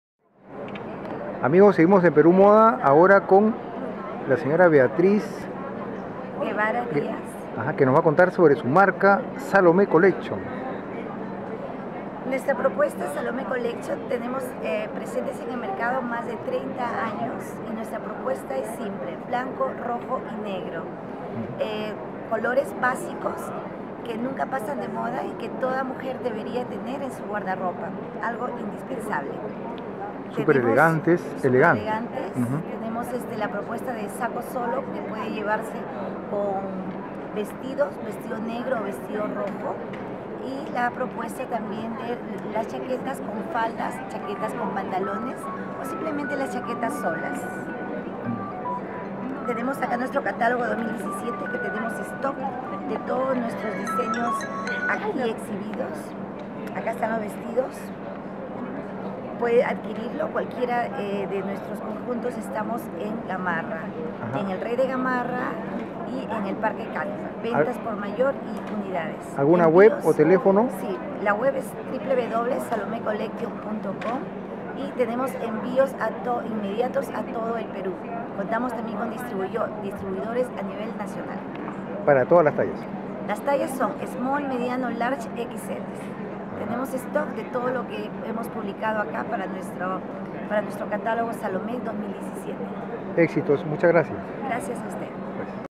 Salomé Collection en Perú Moda 2017. Video reporte.- Javier Espichán
Salomé Collection en Perú Moda 2017
Entrevista con Beatriz Guevara Díaz Gerente General

Revista Mujeres Ejecutivas

Video reporte.- Javier Espichán Gambirazio